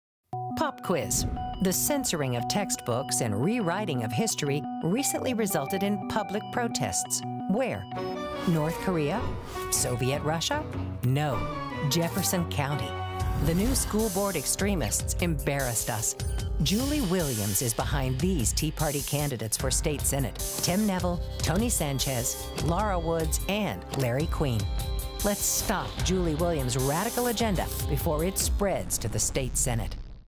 They Embarrassed Us
Julie Williams and the new Jefferson County School Board extremists embarrassed us. Lets make sure her radical agenda doesn't spread to the State Senate. Do NOT support Tim Neville, Tony Sanchez, Laura Woods, or Larry Queen. These Tea Party candidates are too extreme for Jefferson County.